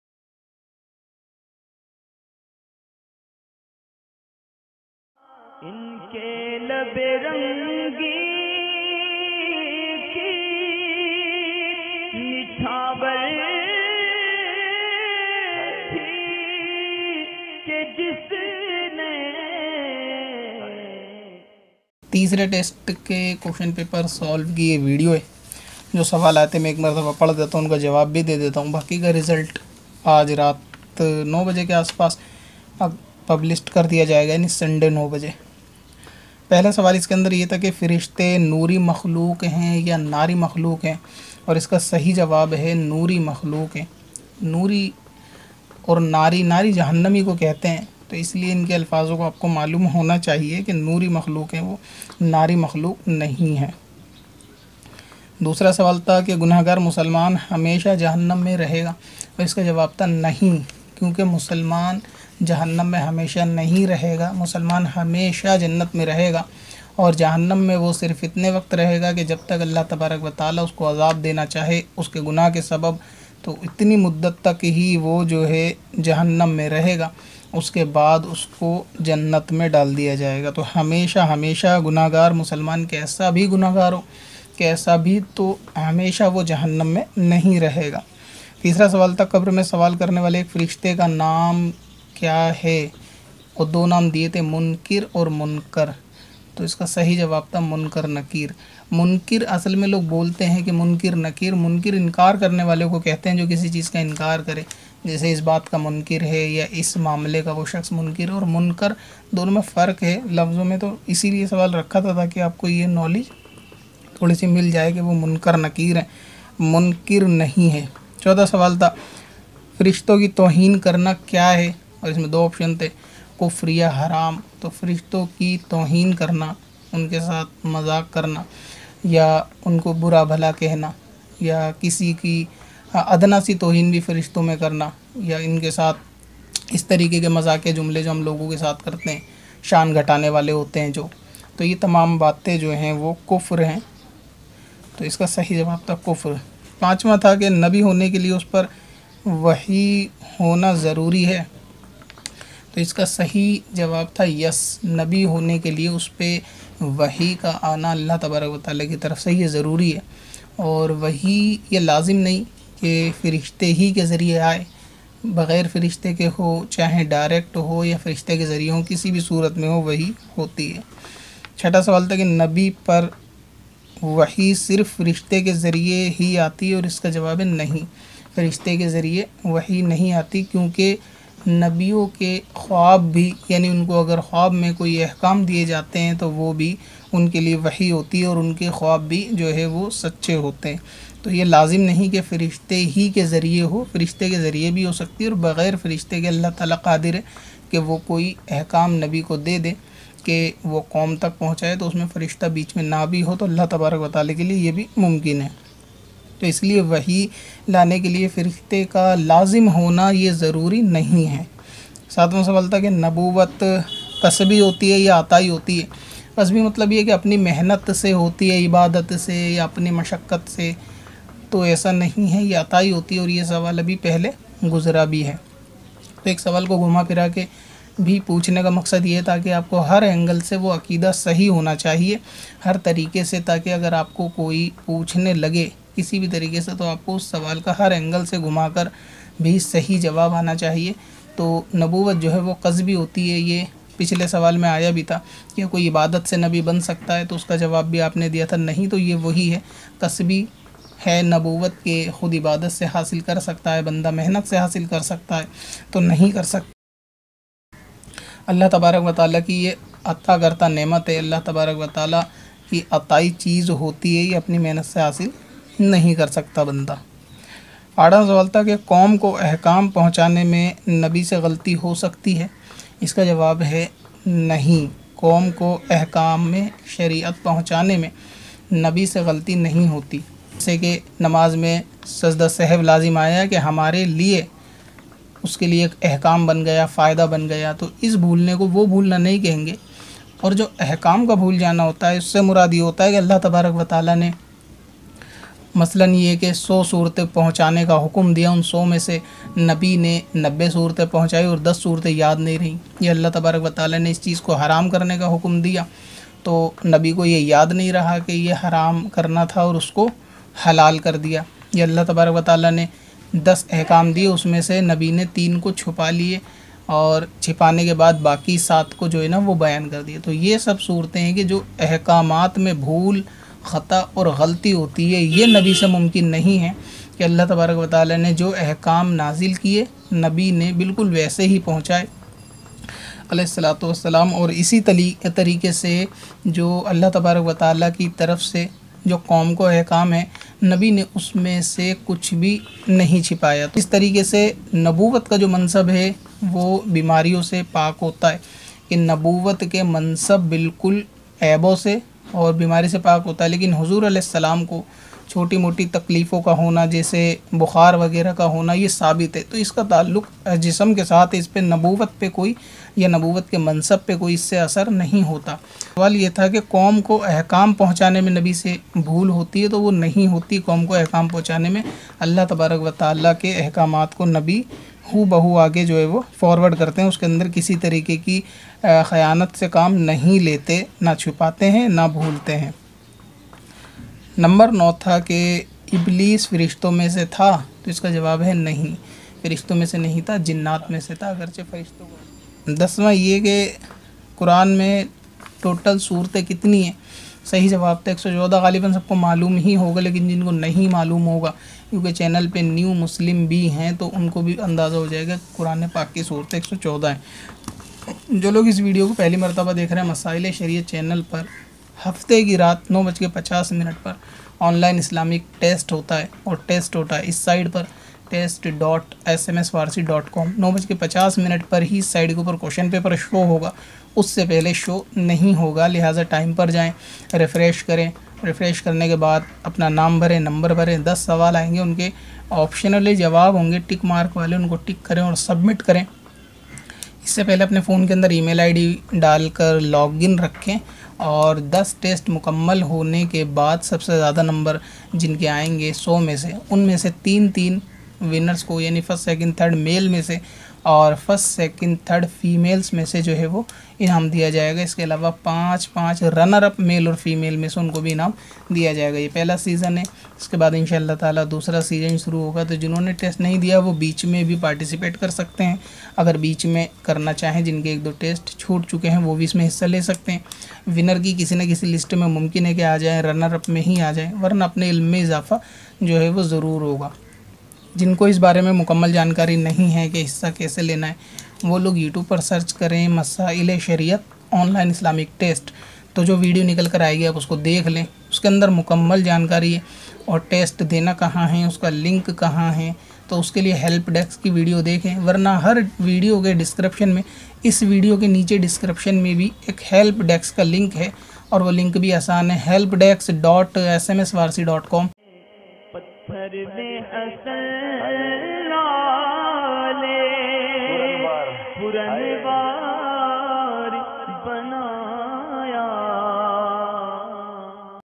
Session 1 Test 3 | Question Paper Answer Video | Masail e Shariat Online Test Result | O.I.T
(NEWS & Update) 📰 📅⏰
Aane Wali Videos & Live Timing Dekhen

masail e shariat online islamic test 3
1. Firishte Noori Makhlooq Hain Ya Naari ?
Noori

2. Gunahgar Musalman Hamesha Jahannam Me Rahega ?
No

3. Qabr Me Suwal Karne Wale 1 Firishte Ka Naam Hai ?
Munkar

4. Firishton Ki Toheen Karna Kya Hai ?
Kufr

5. Nabi Hone Ke Liye Us Par WAHI Hona Zaruri Hai ?
Yes

6. Nabi Par WAHI Sirf Firishte Ke Zariye Hi Aati Hai ?
No

7. Nabuwat KASBI Hoti Hai Ya ATAI ?
Atai

8. Qom Ko Ahkam Pahuchane Me Nabi Se Ghalati Ho Sakti Hai
No

9. Iblees Firishto Me Se Hi Tha ?
No

10. Quran Me Kul Surate Hain
114